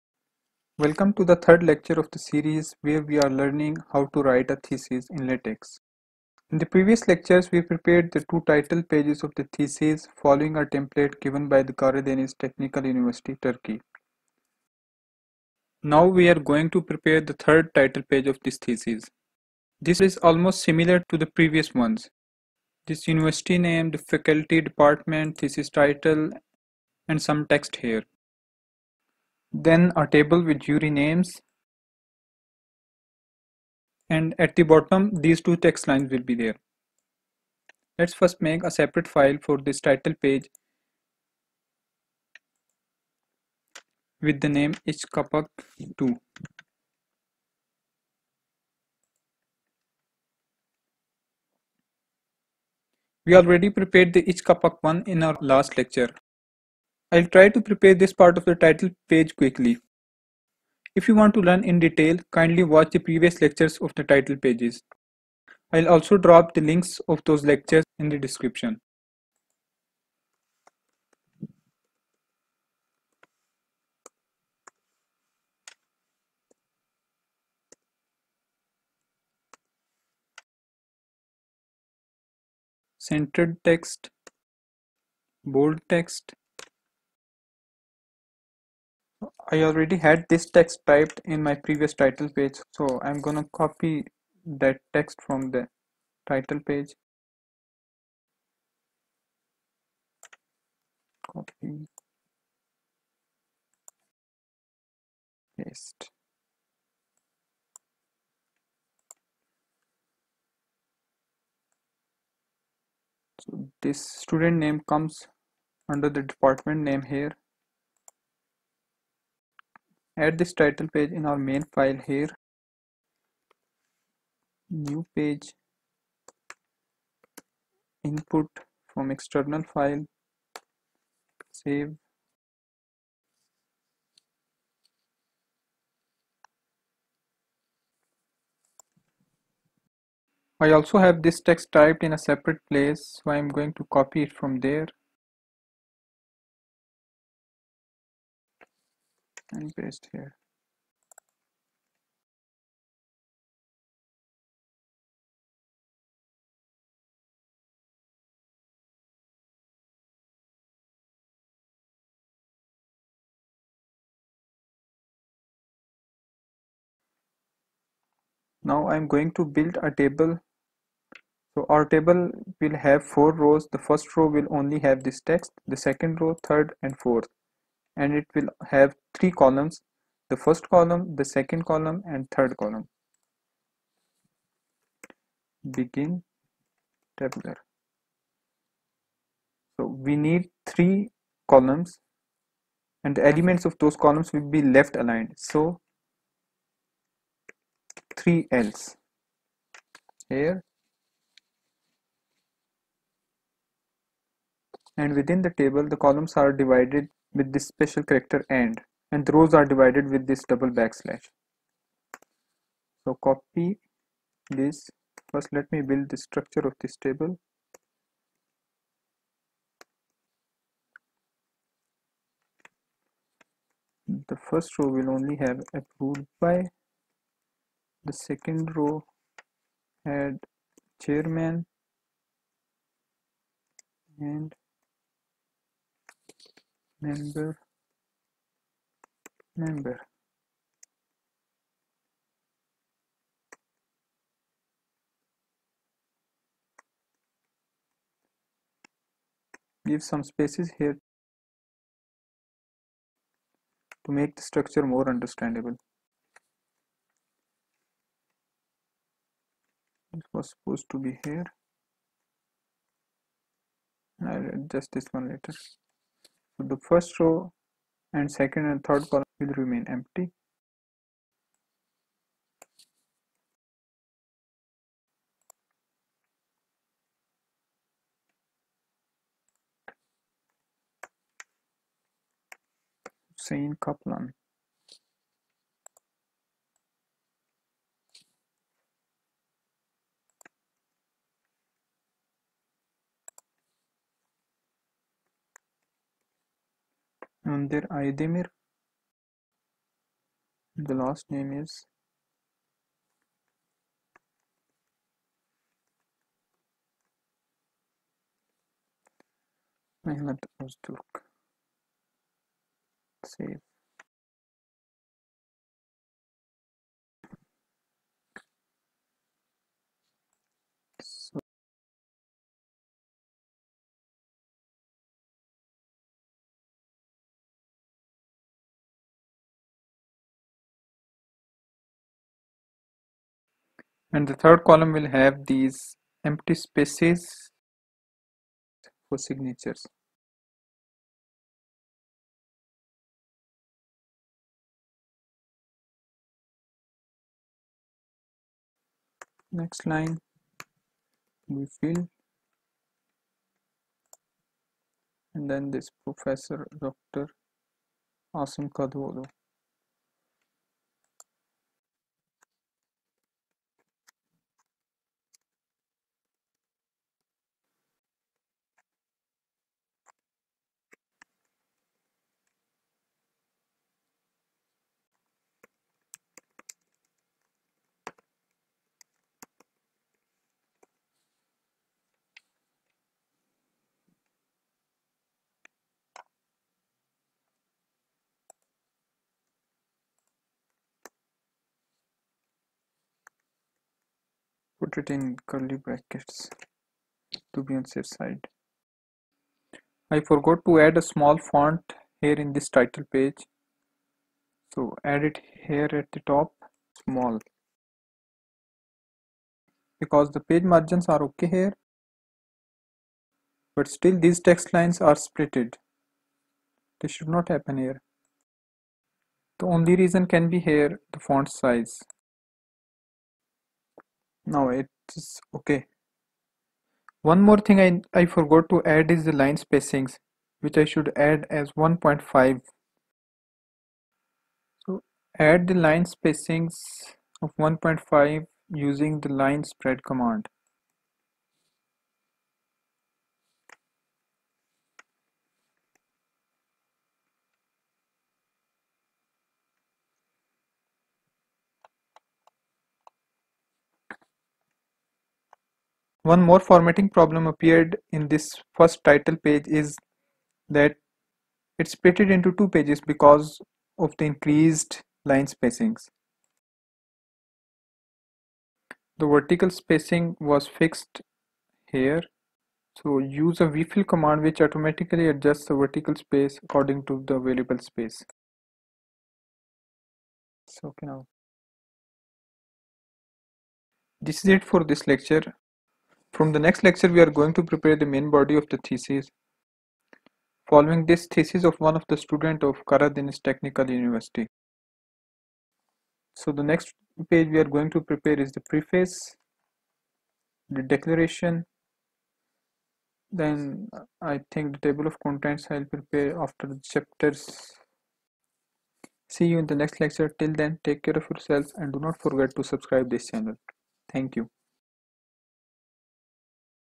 Title Page In LaTeX | LaTeX KTÜ Thesis 3
In this series of lectures you will learn how to write thesis in LaTeX (Overleaf).
In this lecture, the second inner title page of the thesis is prepared following a given template by the Karadeniz Technical University (KTÜ) Trabzon, Turkey. In this lecture, you will also learn how to create a table in LaTeX and how to upload a figure in LaTeX. You will learn vertical spacings using (vspace and vfill) commands in LaTeX.